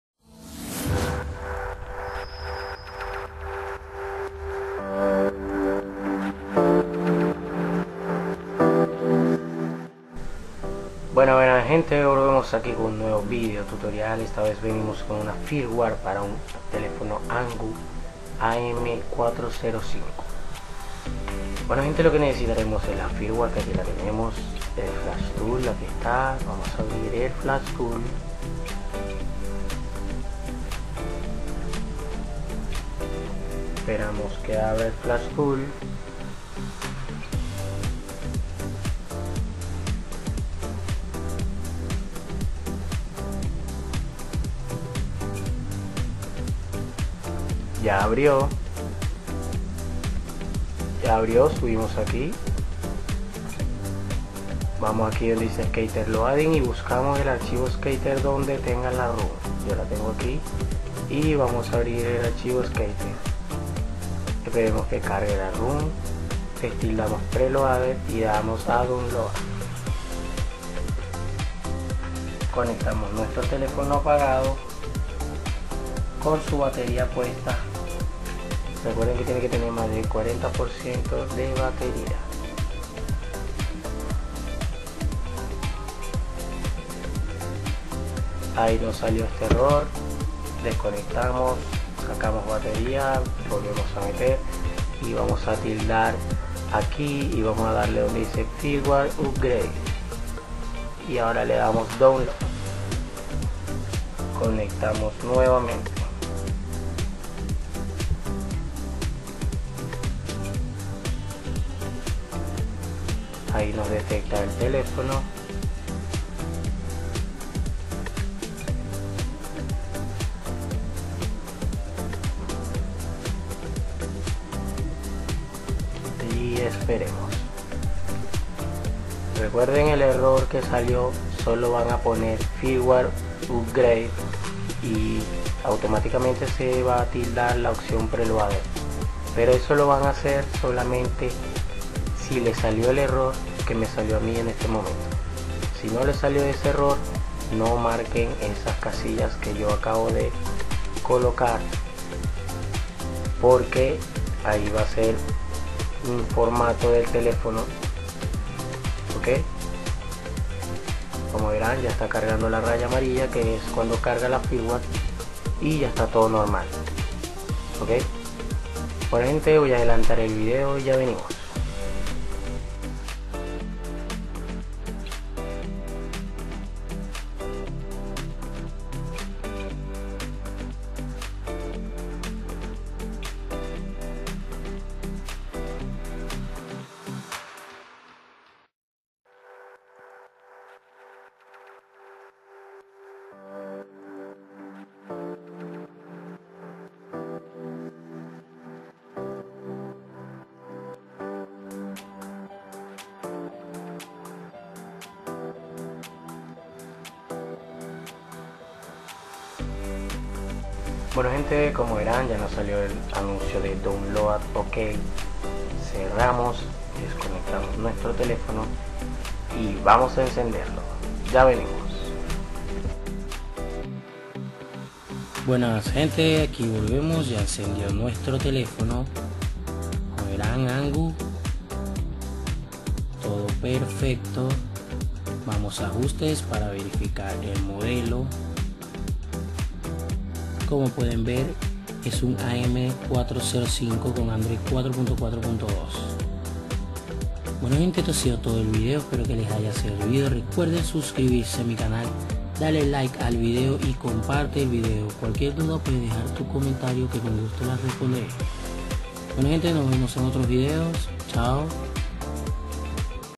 Firmware Amgoo am405 / Software / Desbrick / Solved Done
Buenas gente... aqui les traigo esta Firmware para este modelo Amgoo AM-405.. para aquellos que su telefono se queda colgado en el logo, tiene virus o les anda lento..

Aqui les dejo mi blog, ahi encontraran los Links de descarga..

Liberar Sendtel Rocket Via Imei ( Unlock Phone Sendtel Rocket )